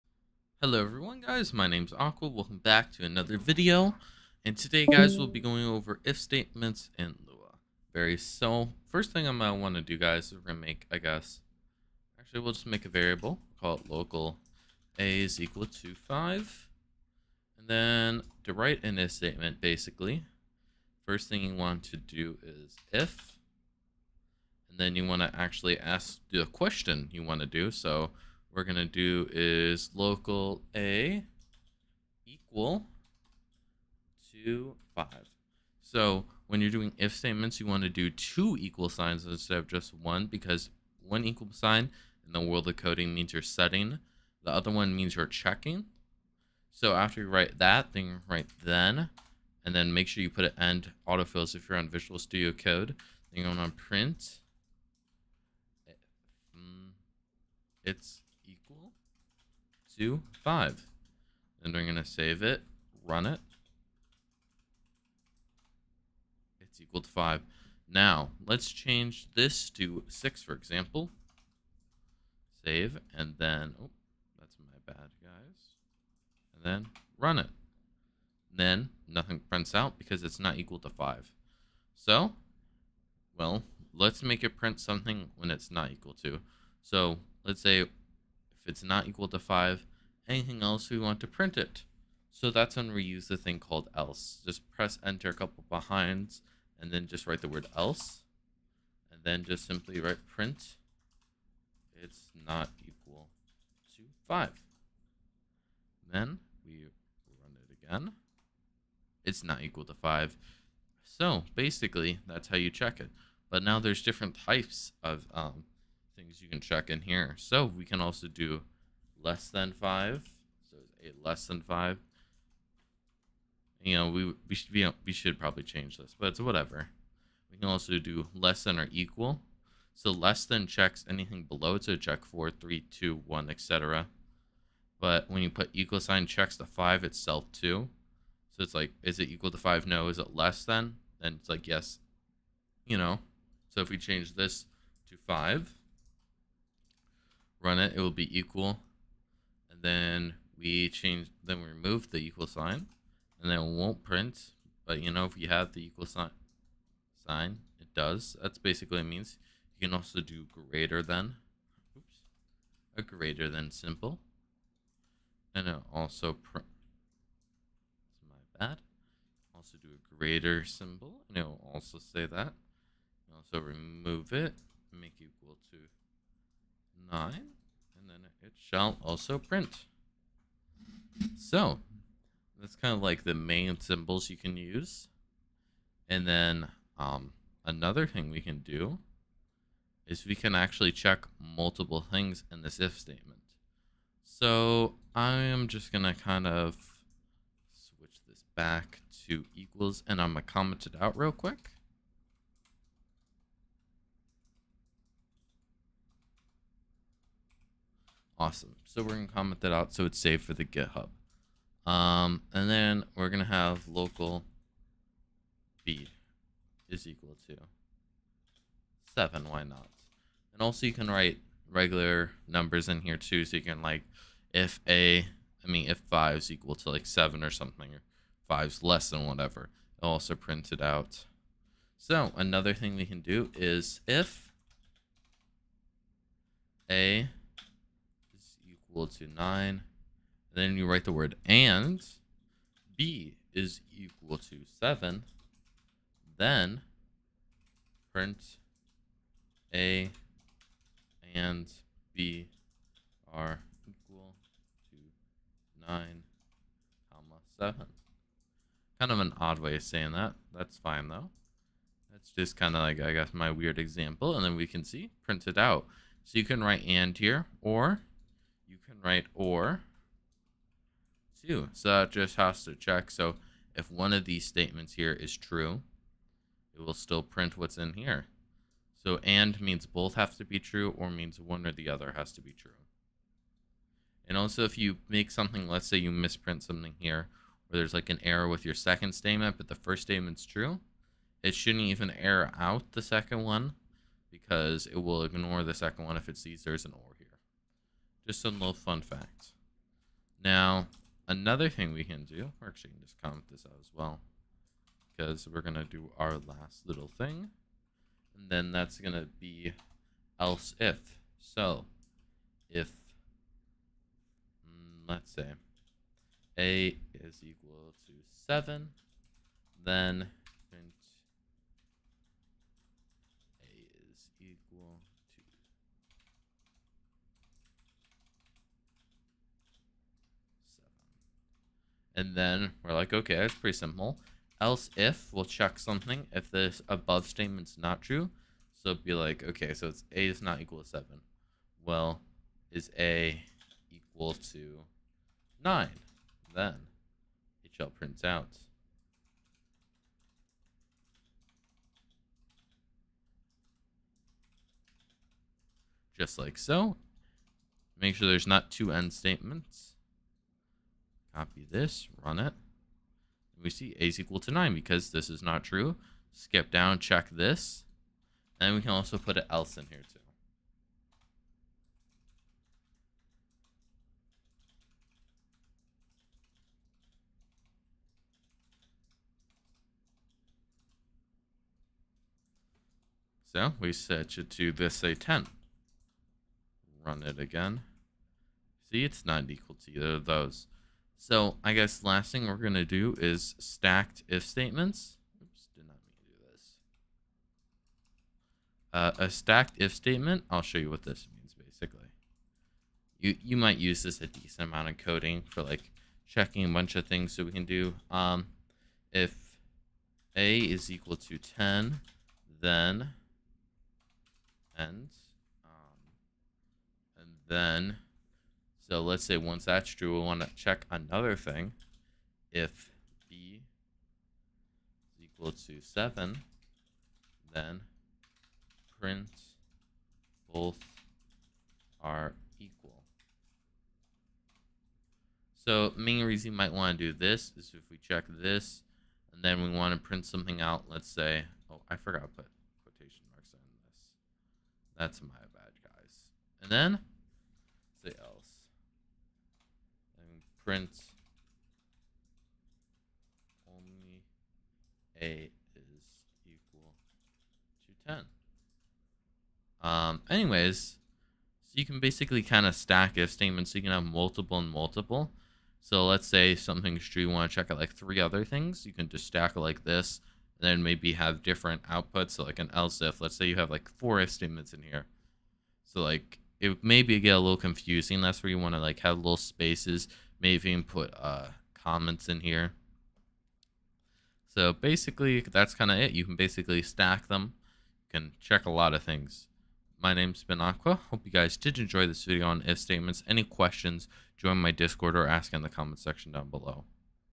Lua. EP 3 If statements
in this video I go over if statements in lua and how to write well made code for Lua